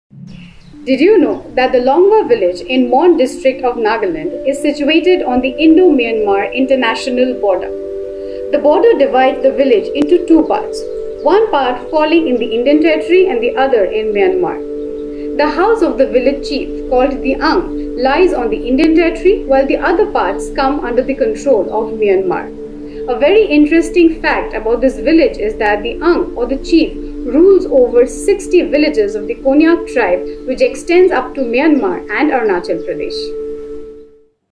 Longwa Village, Nagaland- Did you know! E-20
Did you know about the Longwa is a village that lies 42 km away from the Mon town of Nagaland. Its inhabitants have dual citizenship as the Indo-Myanmar border passes through it.
Please write your feedback and suggestions in the Comments Section!